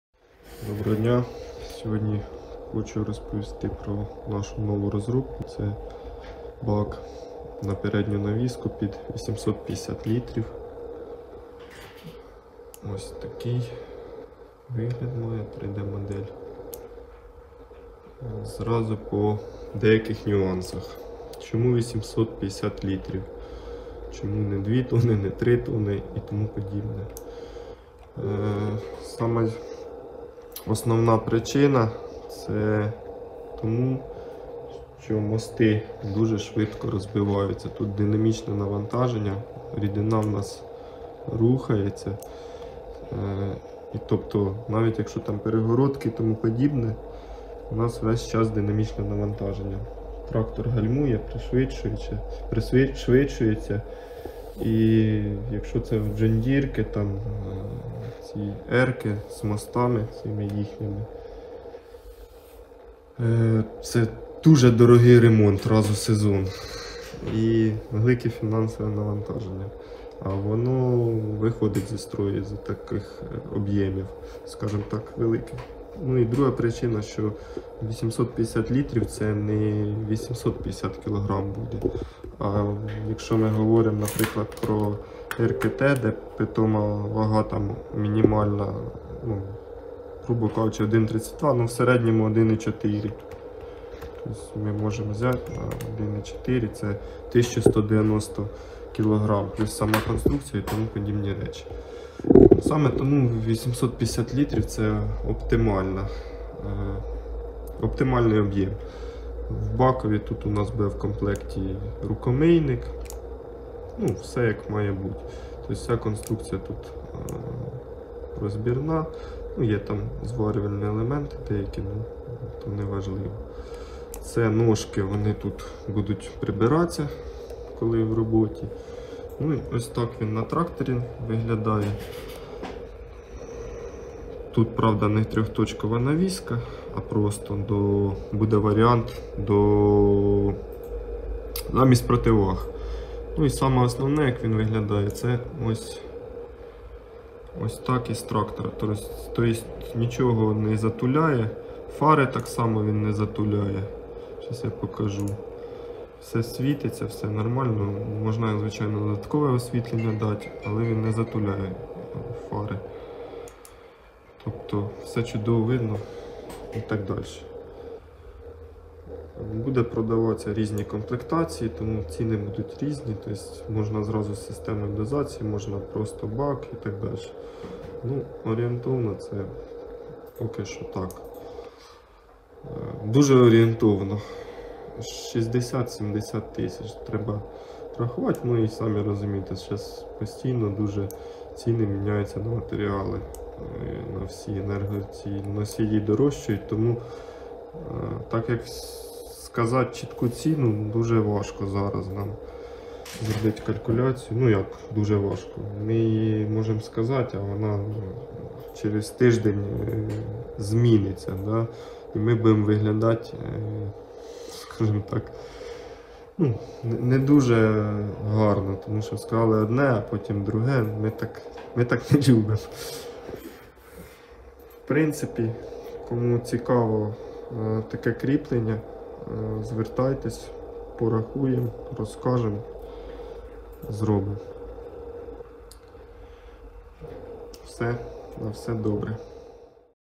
Бак для рідких добрив на передню навіску трактора
Бак для рідких внесення рідких добрив на трьох точкову навіску, об'ємом 850 літрів.